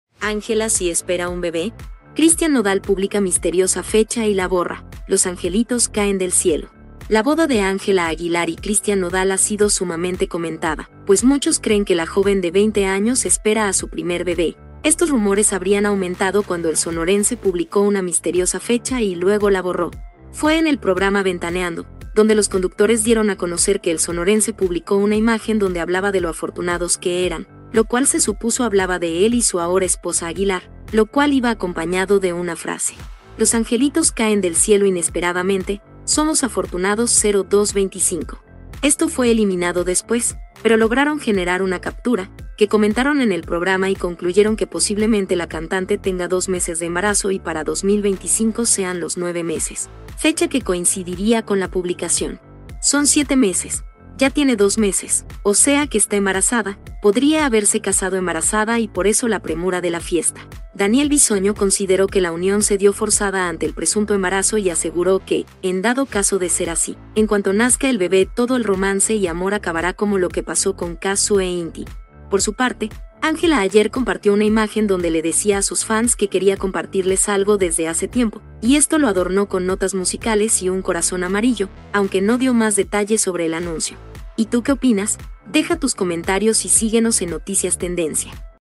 Ángela espera un bebé? Christian Nodal publica misteriosa fecha junto a mensaje y LO BORRA
La boda de Ángela Aguilar y Christian Nodal ha sido sumamente comentada, pues muchos creen que la joven de 20 años espera a su primer bebé; estos rumores habrían aumentado cuando el sonorense publicó una misteriosa fecha y luego la borró.